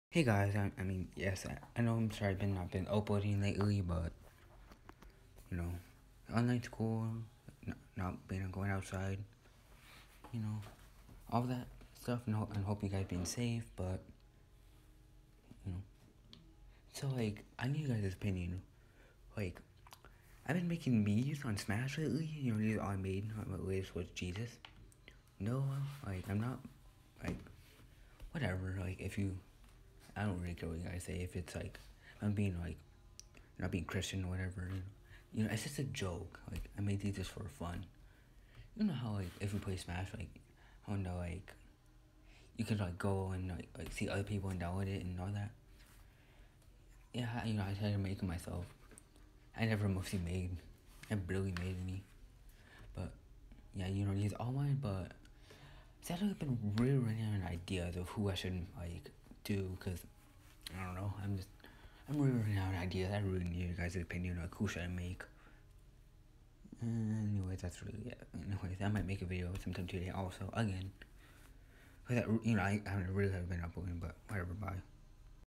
Smash ideas.
Hey, i need smash ideas it can be anything but it has to stabd on two legs and not be weird or whatever.